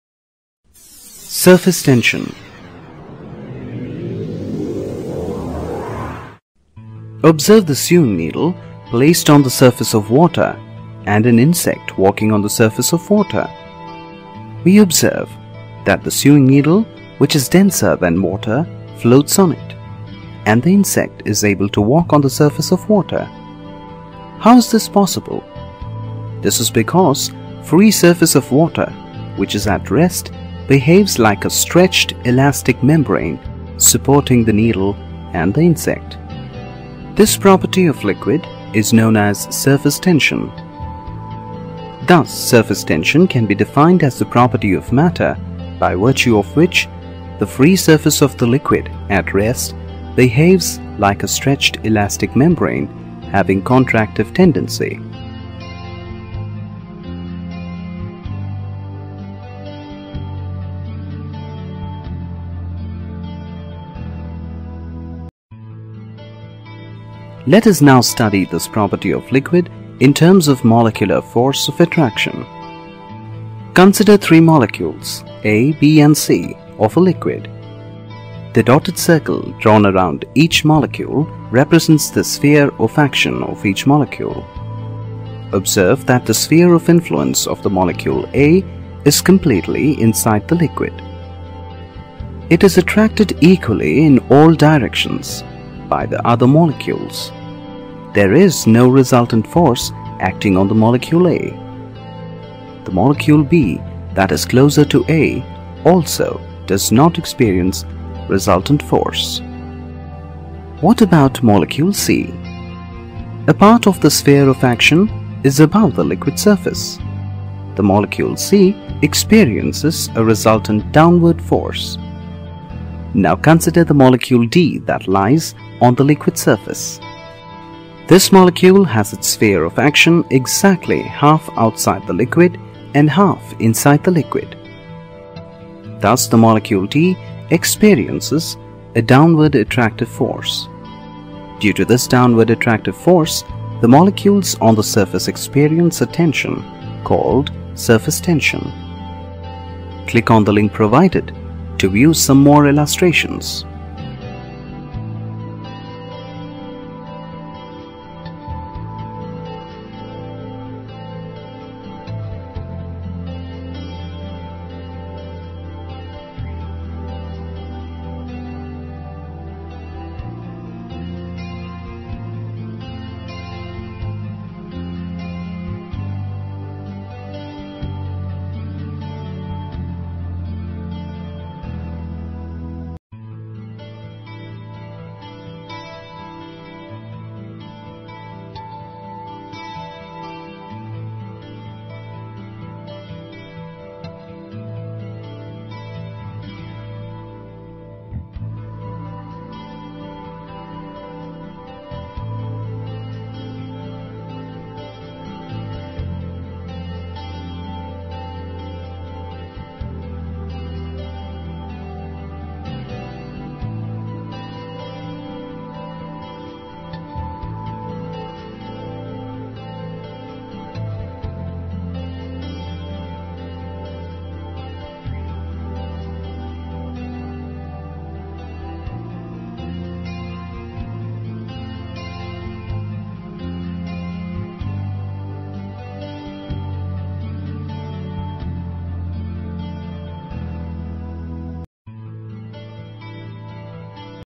Surface Tension - Defintion, Experiment, Observation, Example
What is Surface Tension

Surface tension is the property of the free surface of a liquid at rest behave like a stretched membrane in order to acquire minimum surface area. Surface tension can be defined as the force acting per unit length perpendicular on an imaginary line drawn on the liquid surface, tending to pull the surface apart along the line. Its unit is N m^-1 and dimensional formula is MT^-2.

The property of Surface Tension gives rise to an interesting phenomenon known as capillarity. Capillarity is nothing but the rise of a liquid in a capillary tube. Examples of Capillarity are blotting paper absorbs ink, the oil in the lamp rises up the wick and the sponge retains water.